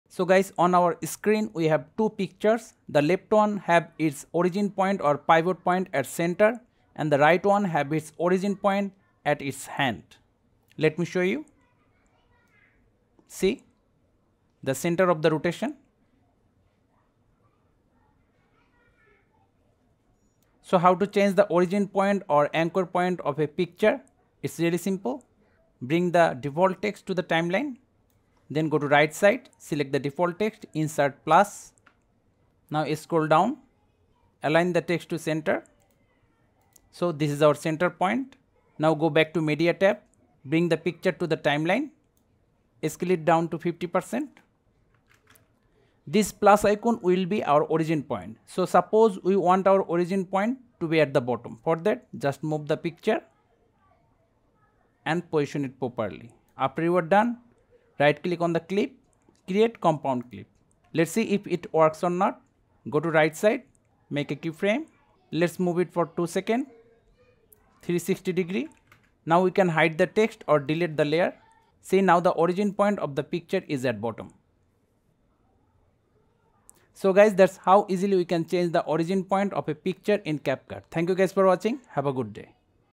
Adjust Anchor or Origin Point | CapCut PC Tutorial
How to change the origin point or center point of a video or picture in CapCut. This is a beginner friendly tutorial on CapCut video editing for Windows and Mac OS users.